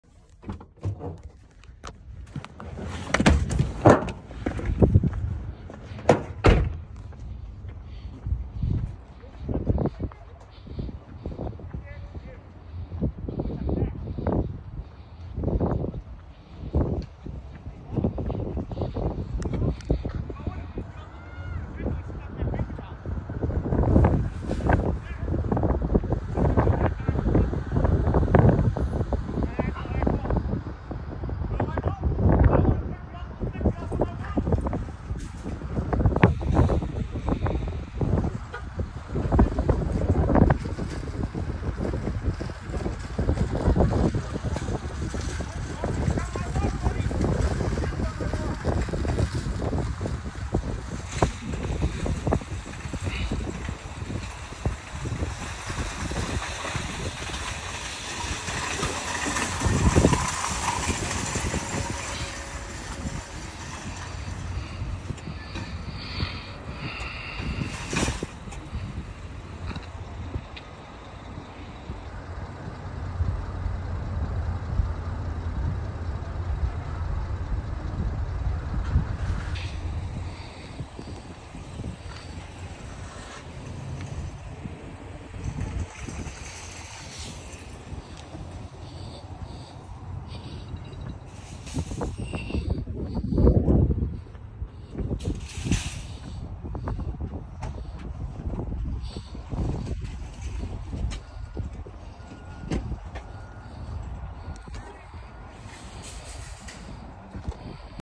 HORIZON CITY TX, WALMART PARKING LOT DRAMA (a man claims he was insulted with the “middle finger”).
ANDREI K. LAGERGREN
HORIZON CITY, TX